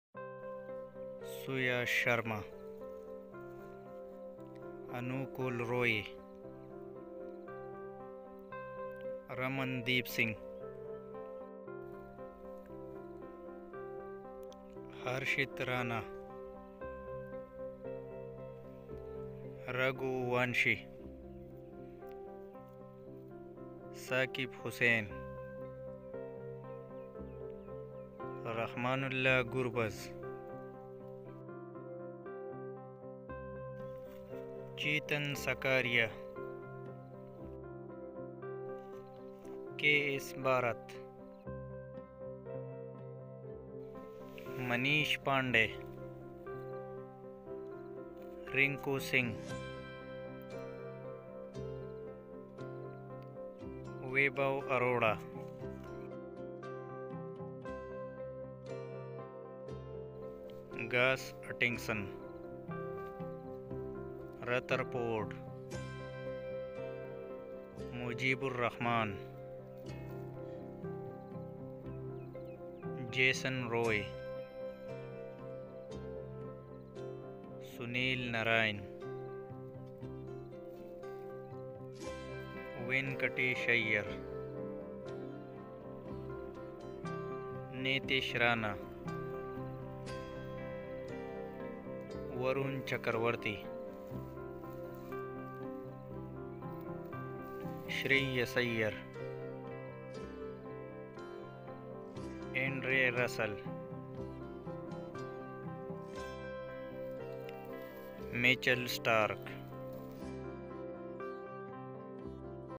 Kolkata Knight Riders IPL 2024 Full Squad with Salaries | KKR Full Squad | IPL 2024 Auction
In this video we shared Full Squad of Kolkata Knight Riders Squad with Salaries for IPL 2024